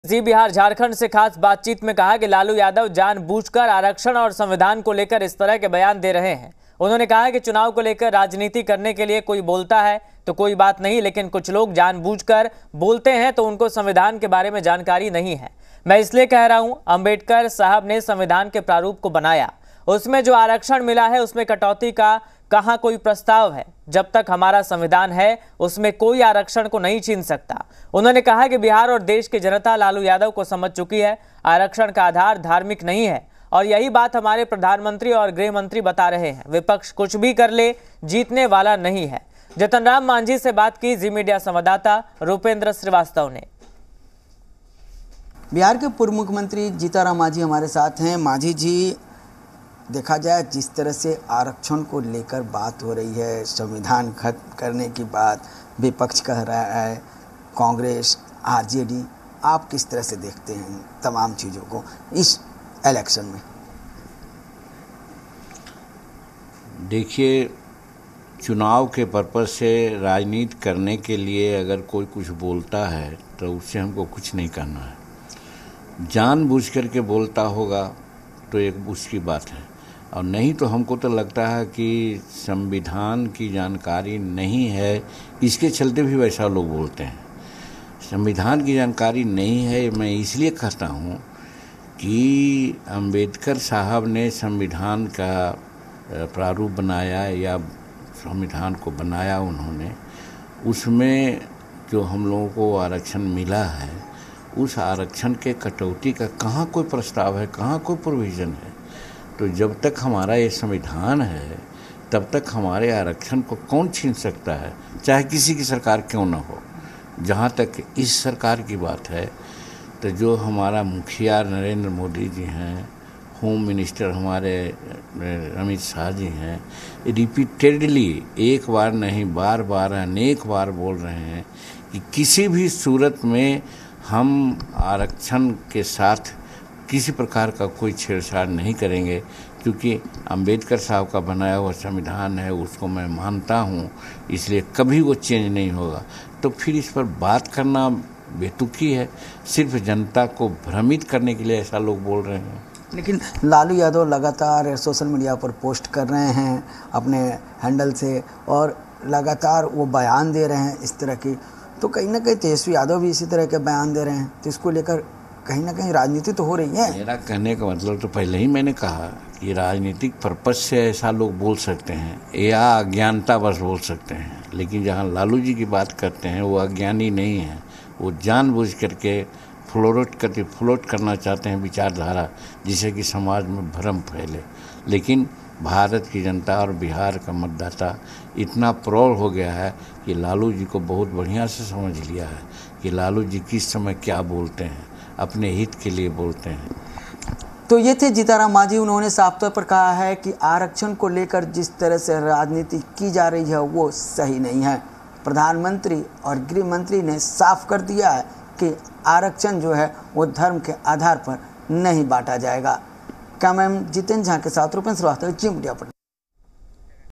Jitan Ram Manjhi On Lalu Yadav: Jitan Ram Manjhi का विपक्ष पर हमला... | Lok Sabha Elections 2024
Jitan Ram Manjhi On Lalu Yadav: Bihar के पूर्व सीएम Jitan Ram Manjhi ने ज़ी बिहार झारखंड से बातचीत की...इस दौरान जीतन राम मांझी दौरान Lalu yadav और विपक्ष पर जमकर निशाना साधा...कहा-'विपक्ष कुछ भी कर ले जीतने वाला नहीं'.